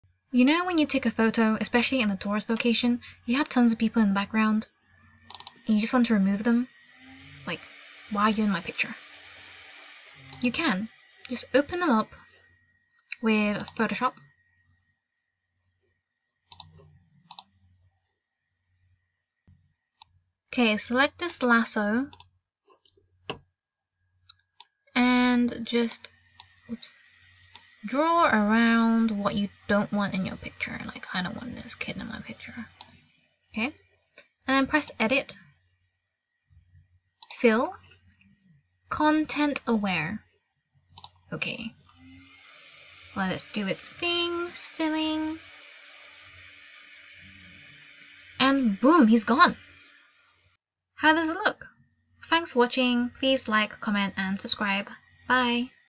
How to remove people in photoshop
Easily remove unwanted people or objects from your photos on photoshop